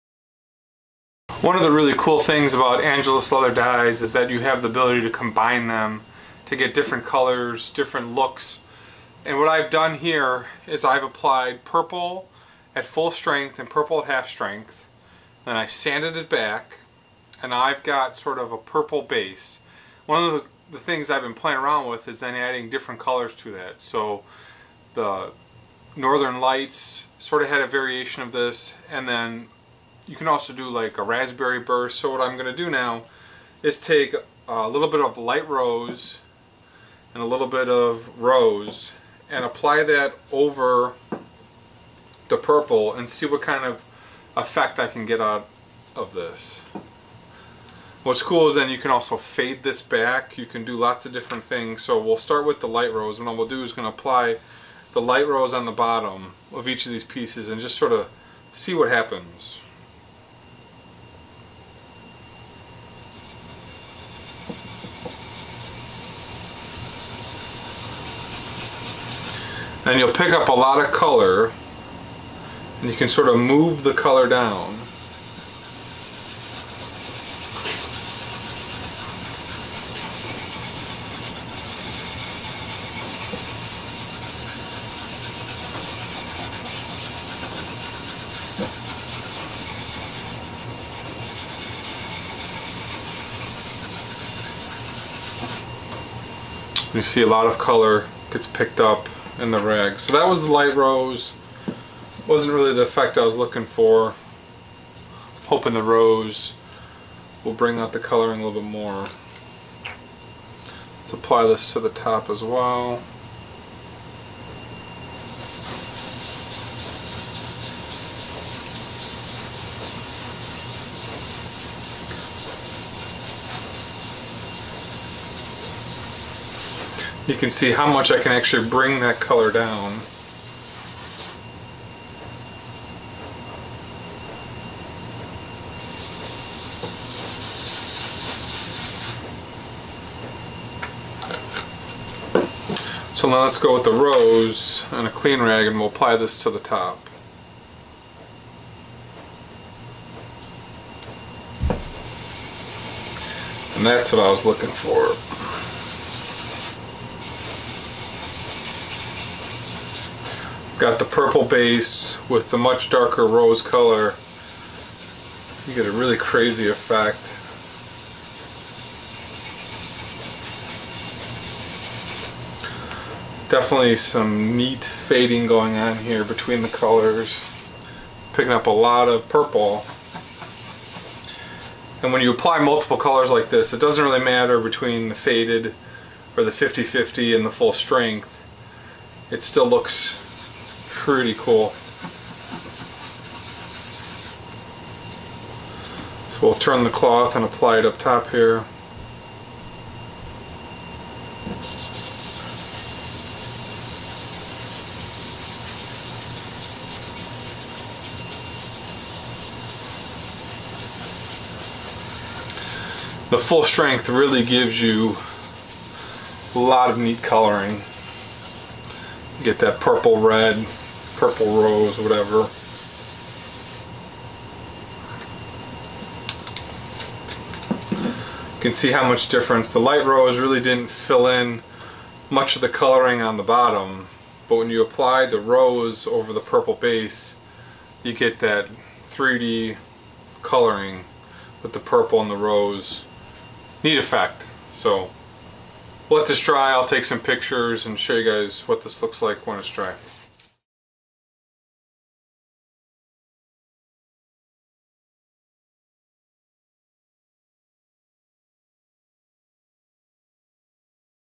Purple & Rose Multicolor Angelus Leather Dye for Woodworking & Luthier Work
Inside the Luthiers Shop with BigDGuitars.com. Going for a color combo today.  Here is a purple Base and Rose.  Really neat effect hard for my camera to take good pictures of this. you can purchase the dyes